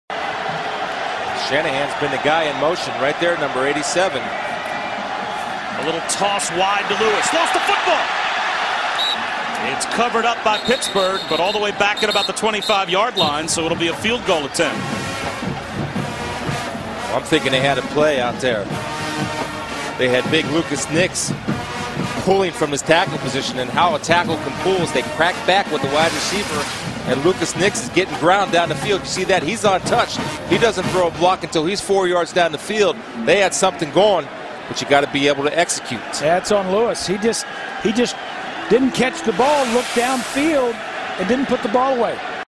2009 Meineke Car Car Bowl, UNC v. Pitt (3Q, play three)
Pitt RB Dion Lewis takes his eye off the ball and turns a possible 3rd down conversion into a field goal attempt.